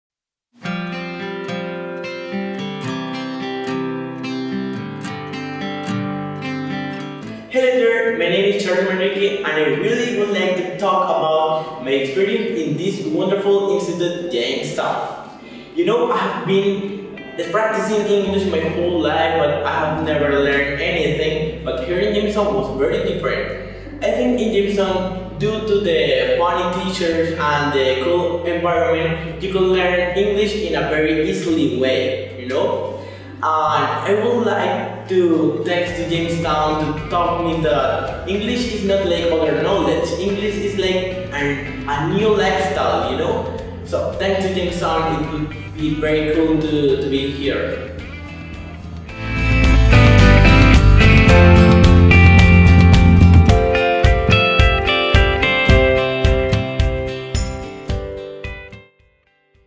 Jamestown English Center, testimonio Charly Chanel Manrique
Elige el programa que se ajuste a tu tiempo y tus necesidades y prepárate con garantías reales de aprendizajes.
jtenglish my new life!